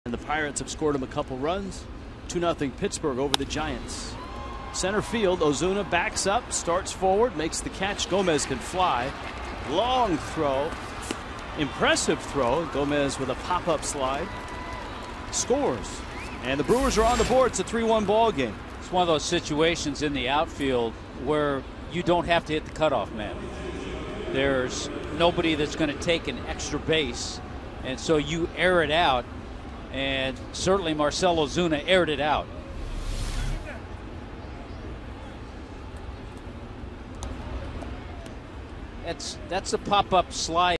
2013/06/11 Lucroy's sac fly
MIL@MIA: Lucroy drives in Gomez in the fourth